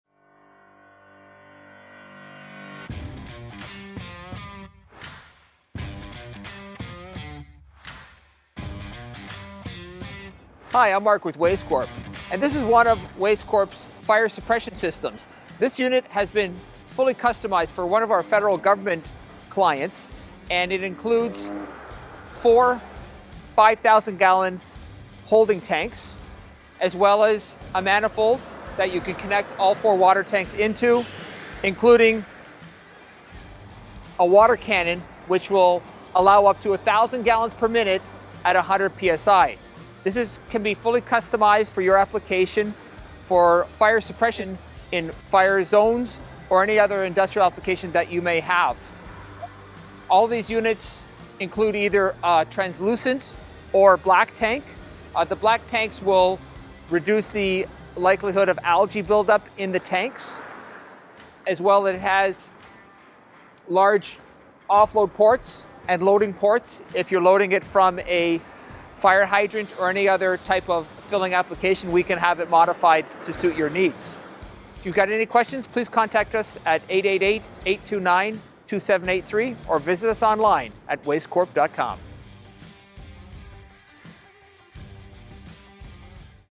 Custom Fire Suppression System by Wastecorp Pumps
Experience the next level of fire protection with Wastecorp's innovative custom mobile fire suppression equipment. When it comes to combating fires, including brush fires and wildfires, our solutions stand as a reliable fortress. Discover the unmatched capabilities of our cutting-edge fire suppression systems, meticulously designed to tackle various fire scenarios with precision.

Featuring powerful water cannons, high-pressure pumps, and advanced valve systems, our mobile fire suppression equipment ensures swift and efficient firefighting. This video serves as your gateway to explore the diverse range of options we offer in the realm of mobile fire suppression.
From the fury of brush fires to the unpredictability of wildfires, Wastecorp's solutions stand as a beacon of reliability and innovation.